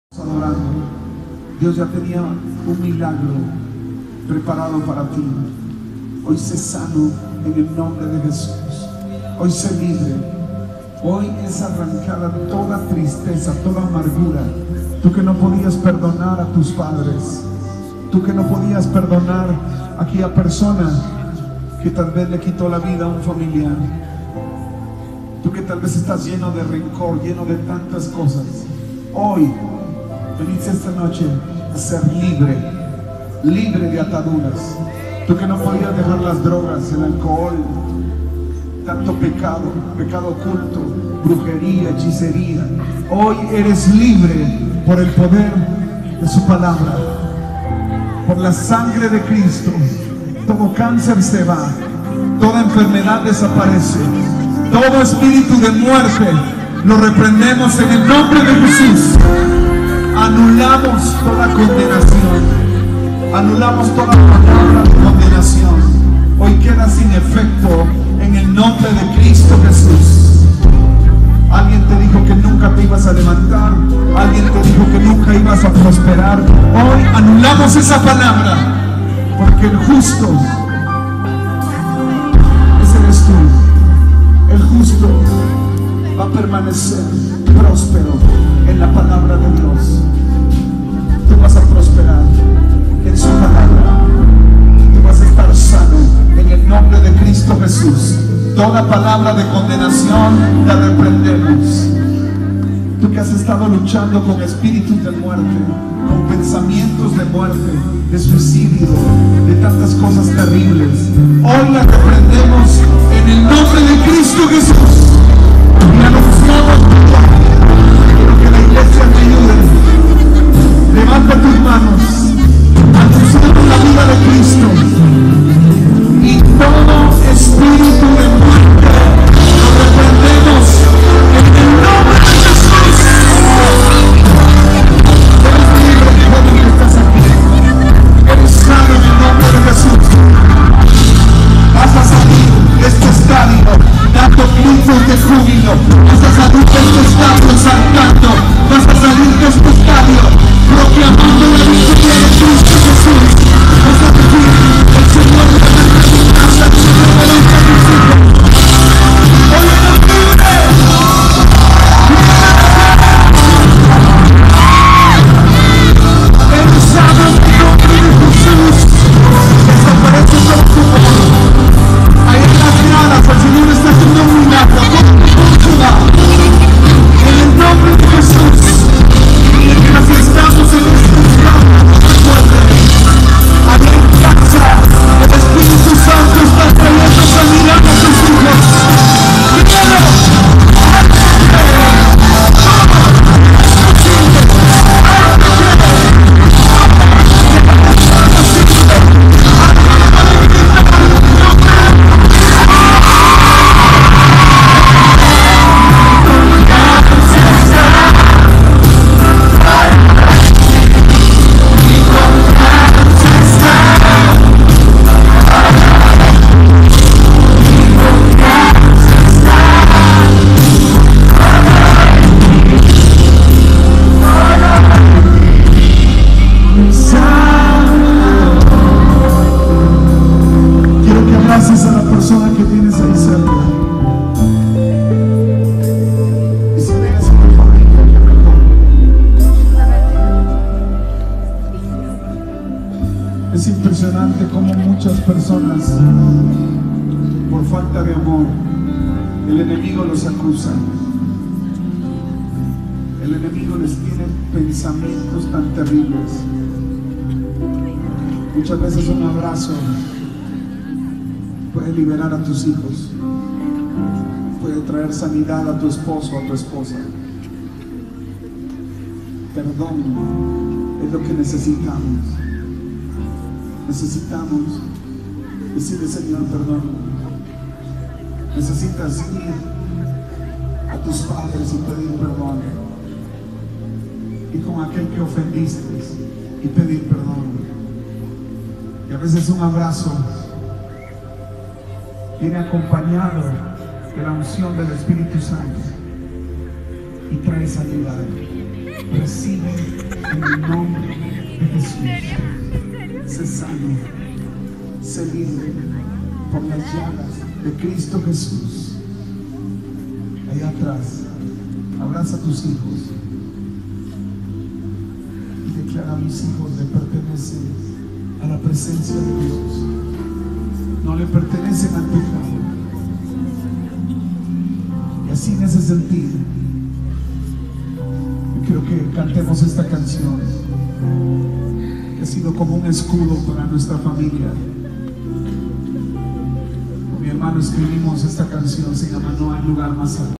MIEL SAN MARCOS CONCIERTO HUANUCO Perú(2017)Y4GR.
Desde la arena Ciudad de Mexico en Huanuco en Concierto 2017
(YG)